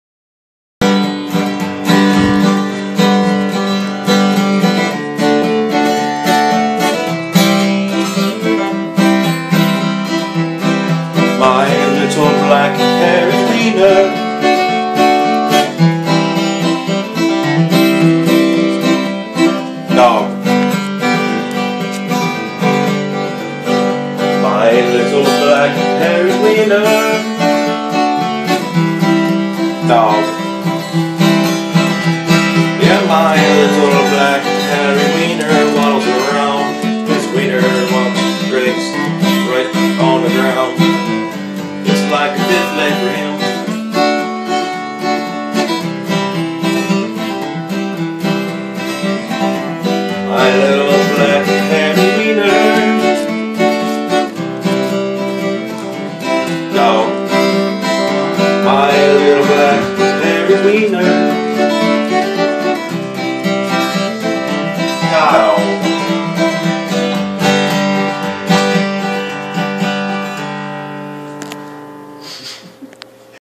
My little black hairy wiener... dog
Cruiser the killer loving the song Trav made for him.